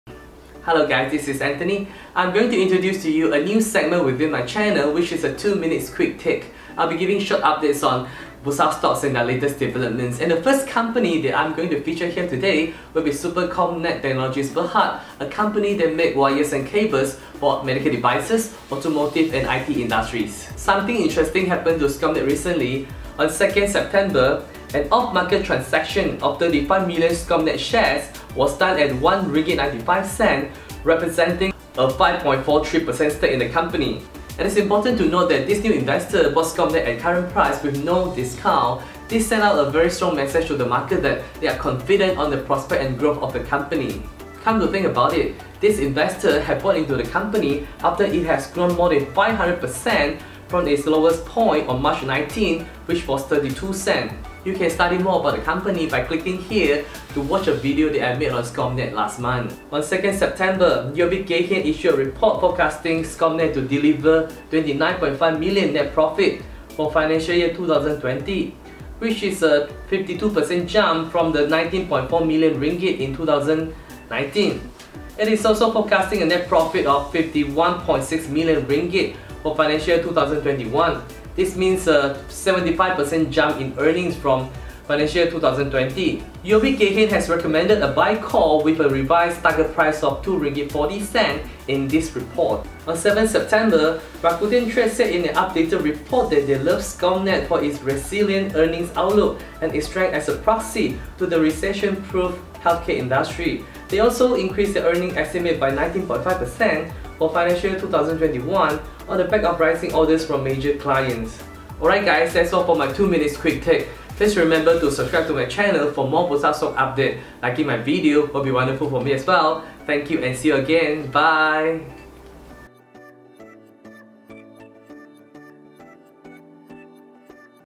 2-min QuickTake | Bursa Stocks - Emergence of a New Investor in Supercomnet?
Presenting to you the new segment within my channel called the 2-minute QuickTake. I will be giving short updates on Bursa stocks and their latest developments.

The first stock in this category will be Supercomnet Technologies Berhad (Scomnet, 0001). A company that makes wires and cables for medical devices, automotive and IT industries.

On 2nd September, an off-market transaction of 35 million Scomnet shares was done at RM1.95, representing a 5.43% stake in the company. This new investor bought Scomnet at the current market price with no discount, which sent a strong message to the market that they are confident with the growth and prospects of the company.

UOB KayHian issued a report forecasting Scomnet to deliver RM29.5mil net profit for FY20, which is a 52% jump from the RM19.4mil in FY19. It is also forecasting a net profit of RM51.6mil for FY21. This means a 75% jump in earnings from FY20.

On 7th September, Rakuten Trade said in an updated report that they love Scomnet for its resilient earnings outlook and its strength as a proxy to the recession proof healthcare industry. They also increased their earnings estimates by 19.5% for FY 2021 on the back of rising orders from major clients.